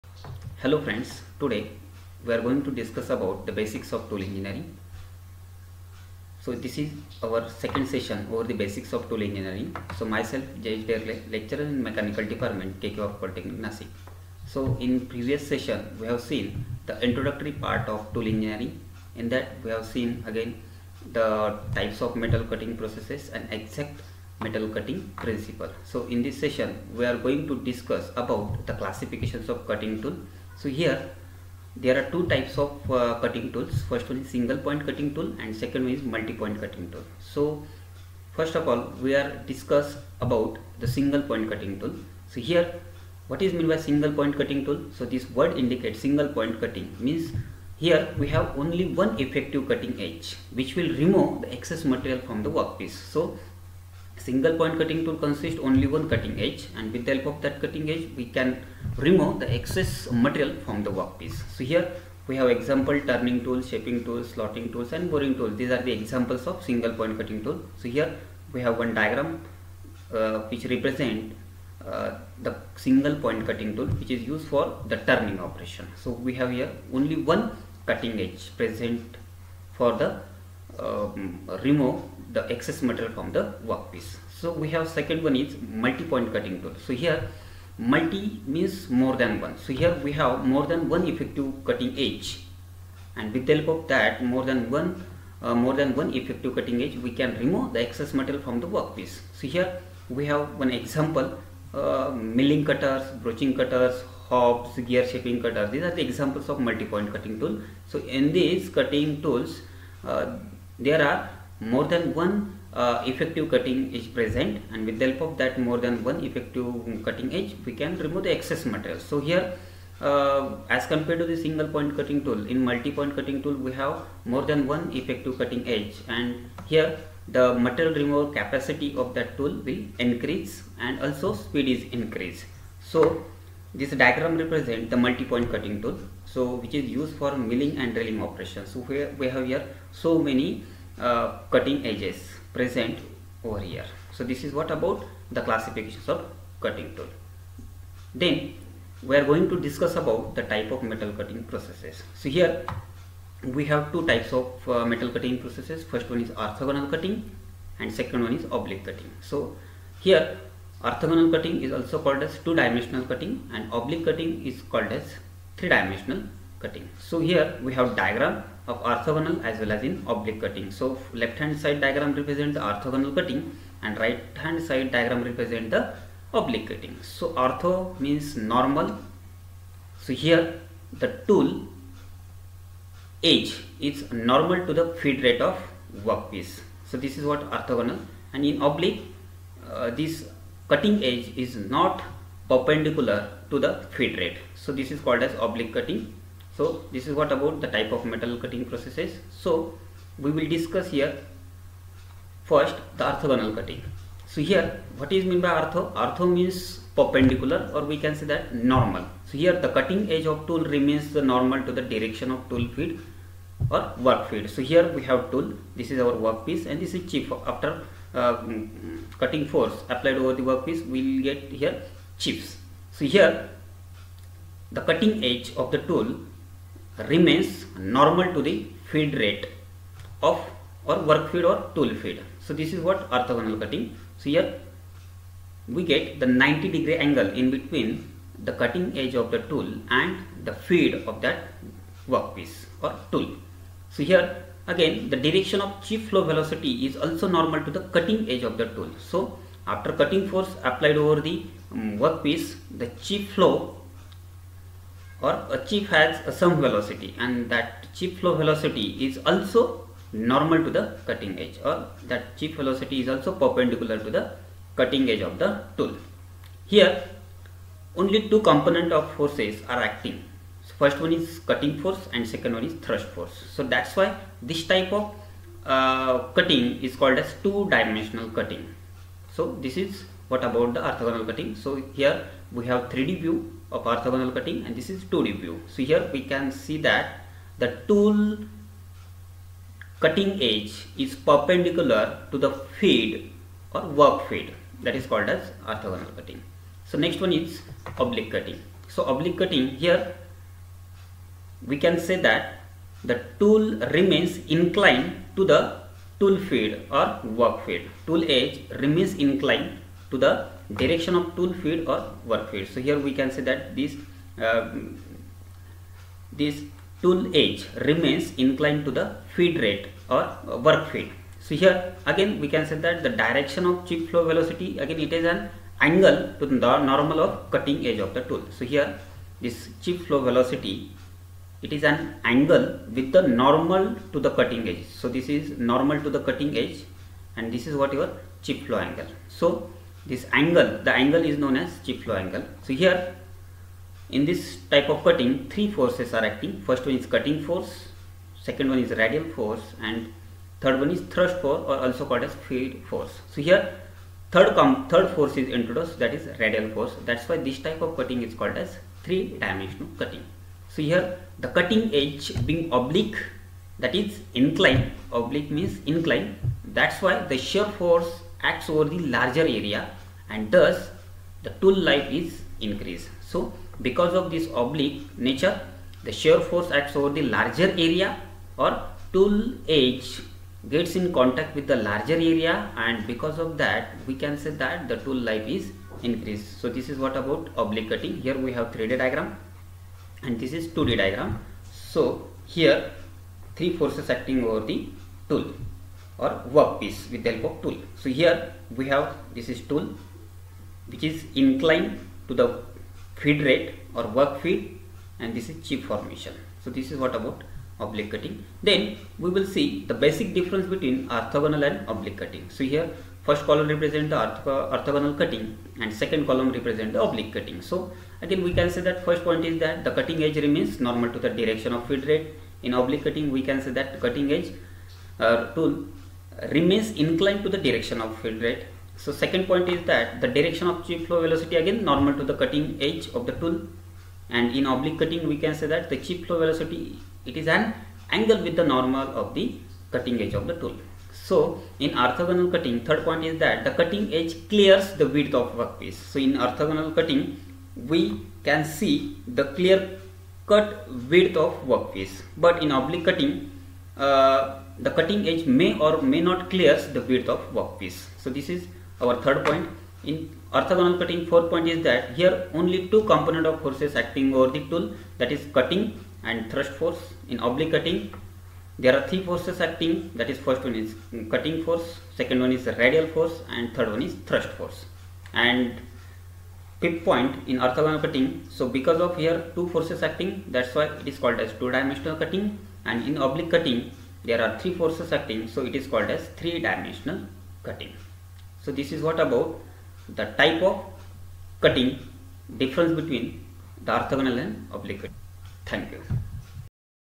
Tool Engineering-Tool engineering Introductory lecture for Diploma and Competitive exams-Part -02
Students will able to understand types of cutting tool, type of metal cutting process,their differences